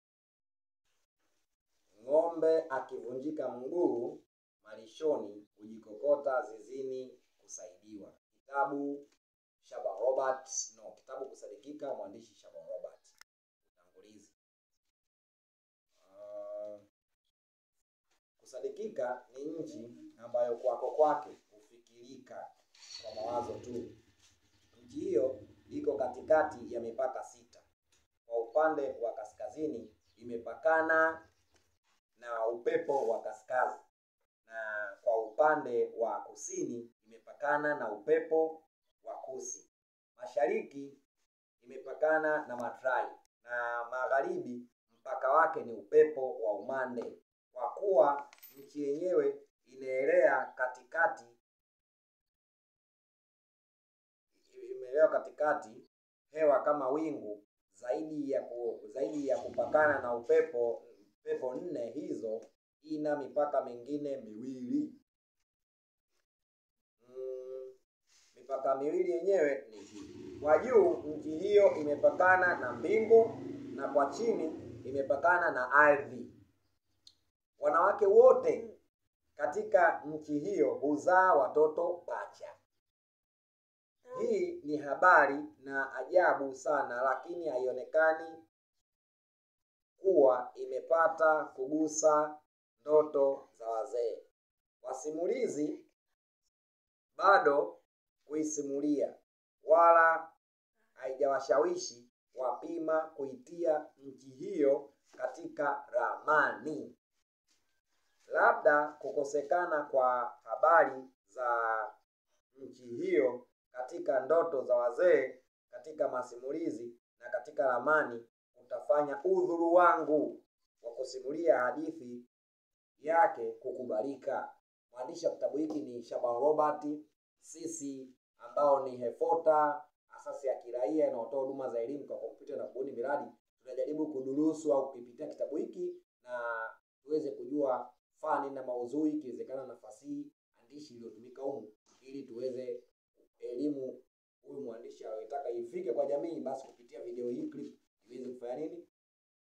KITABU KUSADIKIKA MWANDISHI SHABAN ROBERT
KUSADIKIKA ni kitabu chenye maudhuni mapana kuhusiana na umasikini na namba ya viongozi wanavyotakiwa kubadilika kulingana na nyakati